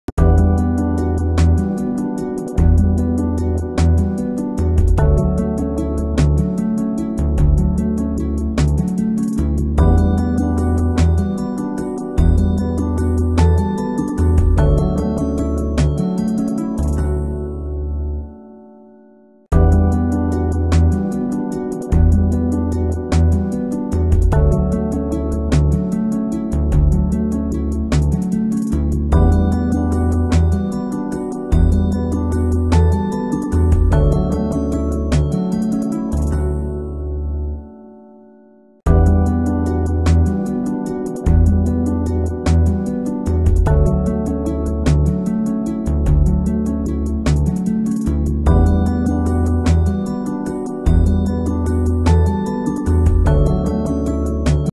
Lovely circle💕🎇|wall hanging craft💕
Materials used:- color papers
                              Hot glue
                              Tin cardboard
                             Cello tape
                             Colors
So do try this craft and share your feedback too.
 I have shared other craft videos in my channel you can also check these easy And simple craft videos